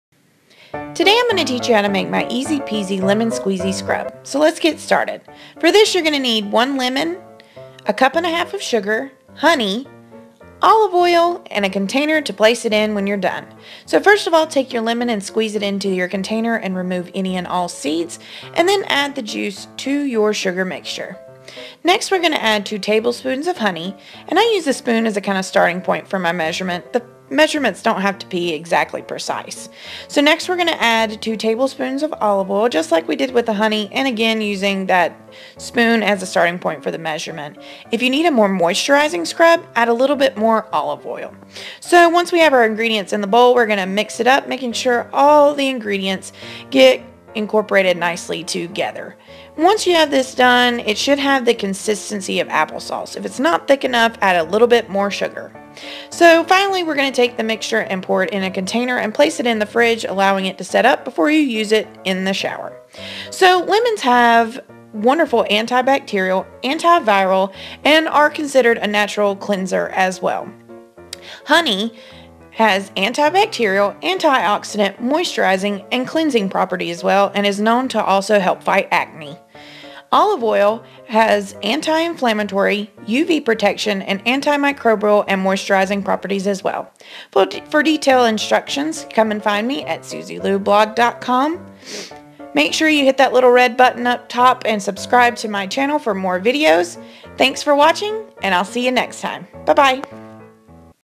Easy Peazy Lemon Squeezy Scrub
Follow along as Suzie Lou Blog Creator, Laura Rhinehart, teaches you how to make her Easy Peazy Lemon Squeezy Scrub!